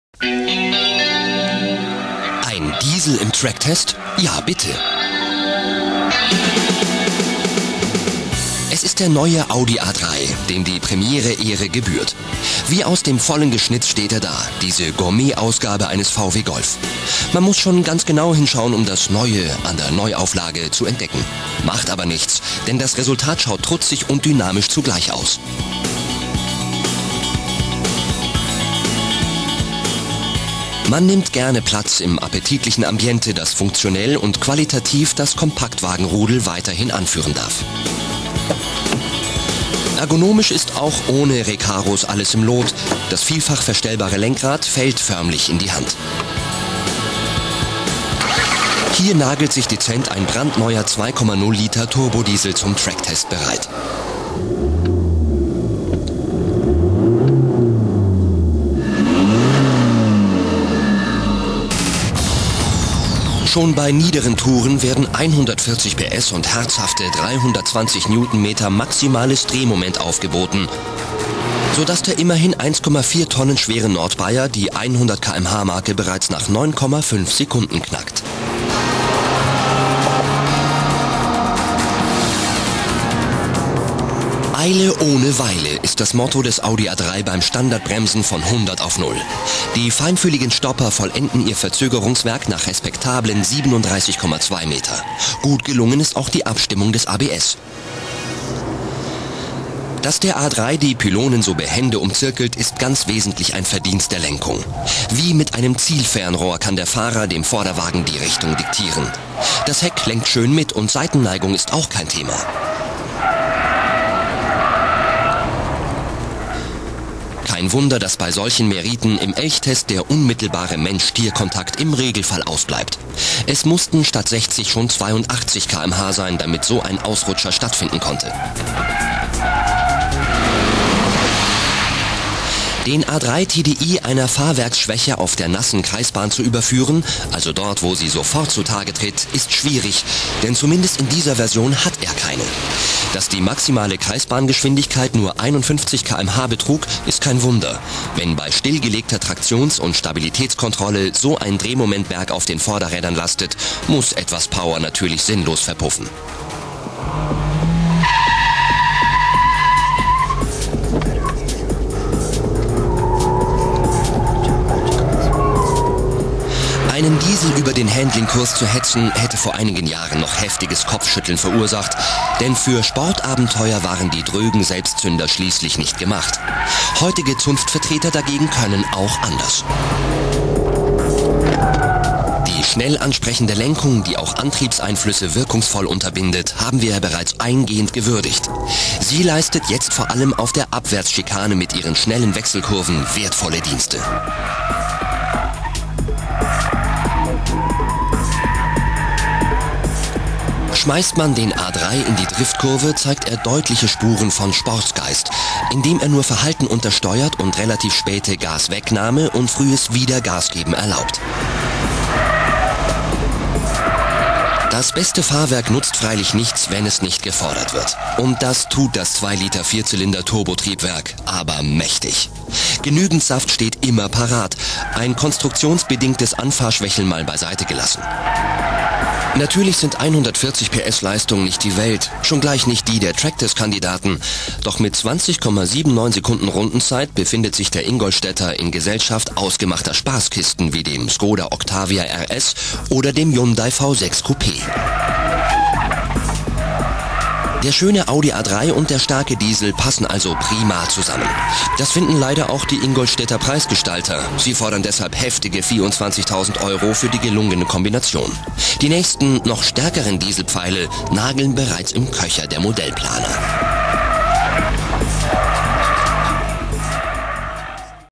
Test Audi A3 2.0 TDI 140
Tracktest in tedesco dell'Audi A3 TDI da 140 cv